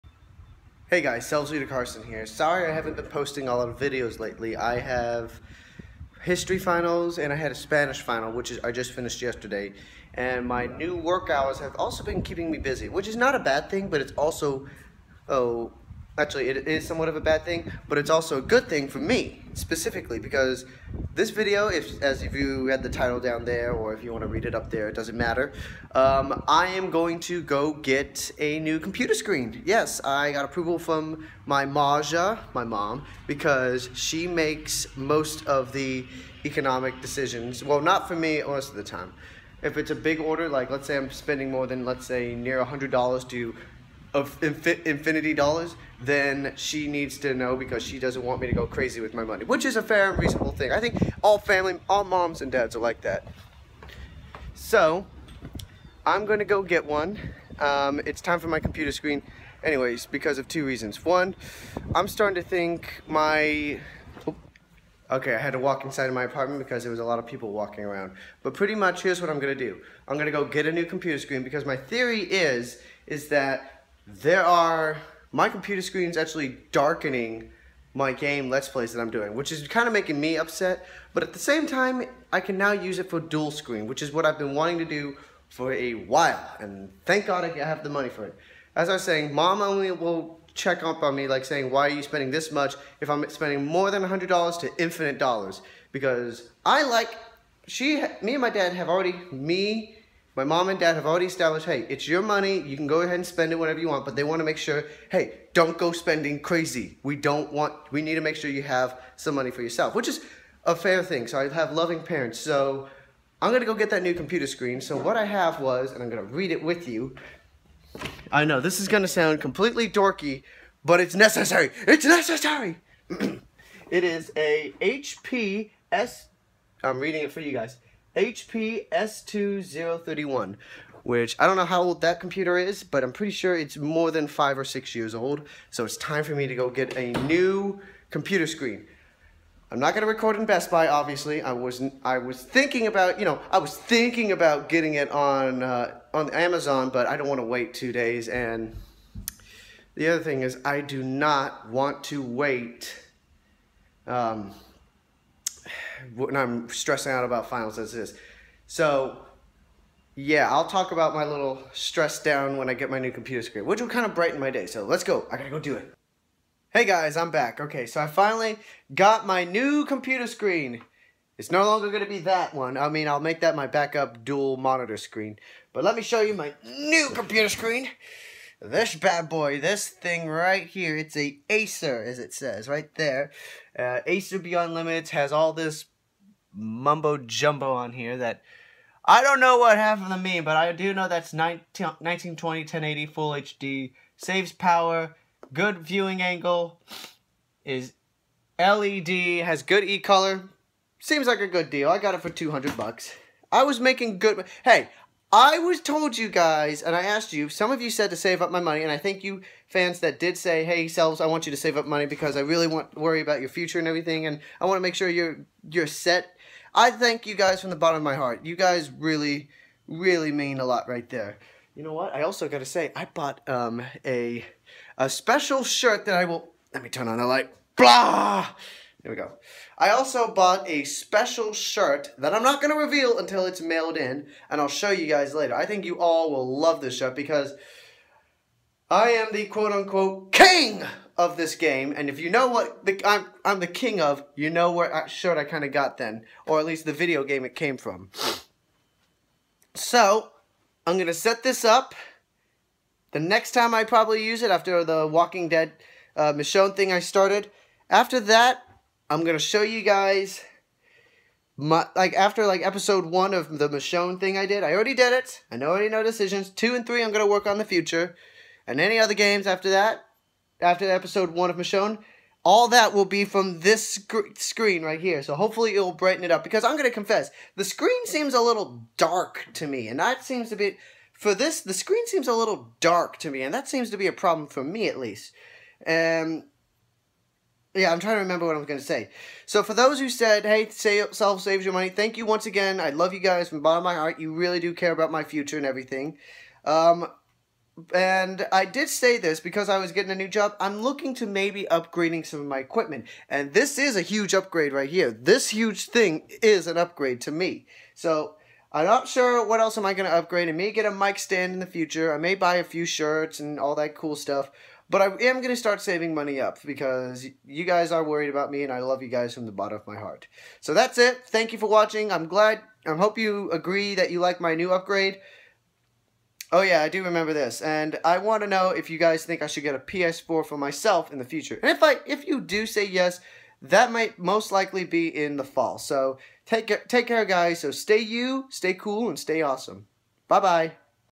SelvsVlog #26 - I'm Getting A New Computer Screen! :D
Hey guys! I thought I should let you all know this but with the extra EXTRA money I've been making, I'm actually going to upgrade one of the things I've been wanting to upgrade and that's a new computer screen! I'm super happy and hope you guys like it!

BTW, Sorry for not making videos lately, College finals have been slowly gnawing at me, and I'm stressing out over them. NOT TO WORRY!! I'll still have them done and with good grades! :D Stay Awesome!